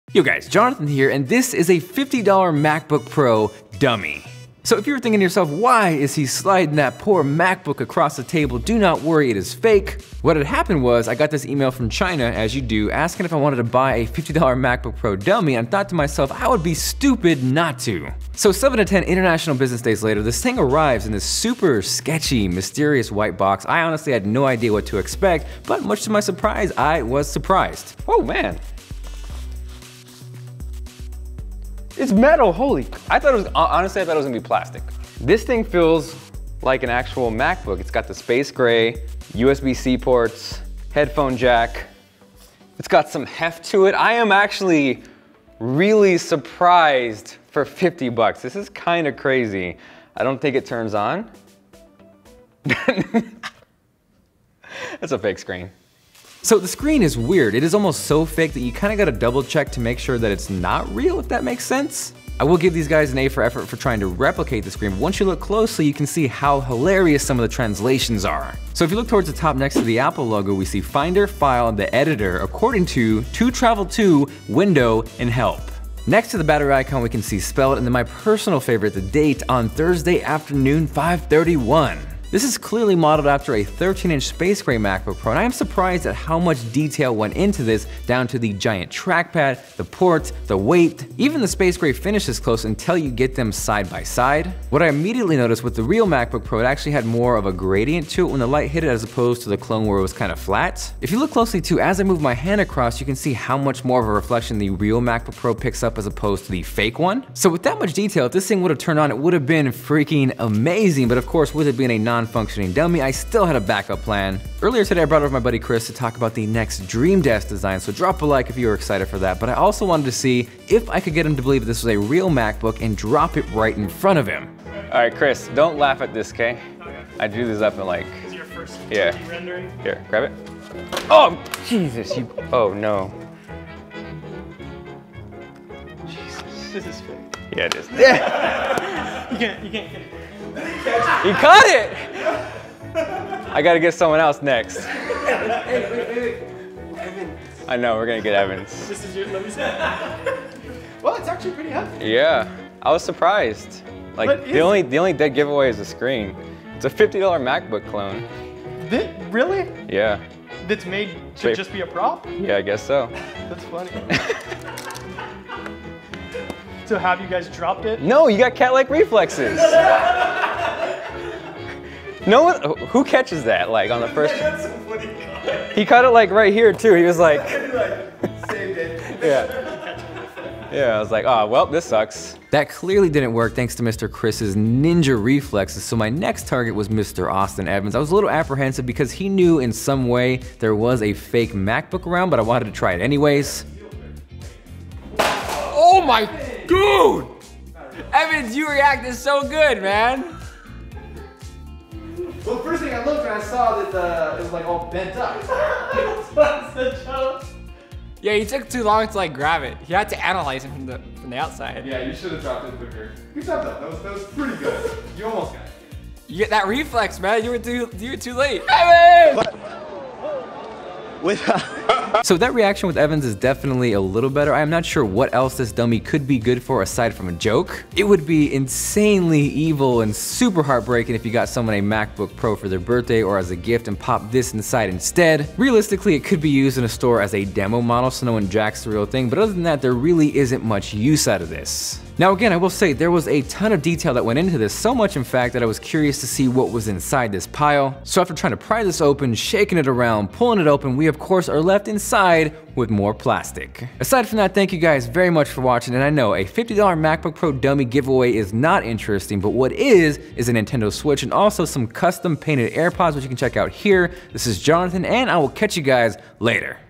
$50 Fake MacBook Pro vs $1,500 MacBook Pro
Found a $50 MacBook Pro dummy but how does it stack up vs a real $1,500 MacBook Pro?